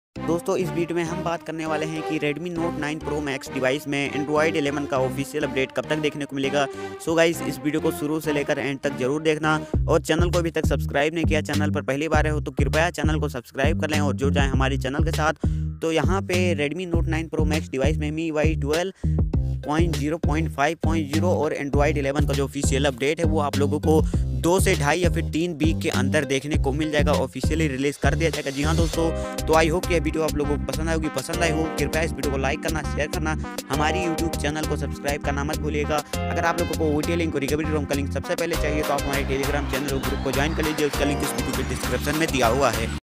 Redmi Note 9 Pro Max New Update | Android 11 Redmi Note 9 Pro Max | 12.0.5.0 Redmi Note 9 Pro Max
Hello friends eash video me hamne apko bataya hai ki Android 11 ka OTA stable Update 😍 Redmi Note 9 Pro Max me kab tak aayega kab tak rolling out hona start ho jayega agar aap ko koi bhi problem hai to jarur comment kare

Jai Mahakaal | Jai Mahakaal | Jai Mahakaal | Jai
Jai Mahadev | Jai Mahadev | Jai Mahadev | Jai

Search On Telegram 👉 Technical Shivam Ji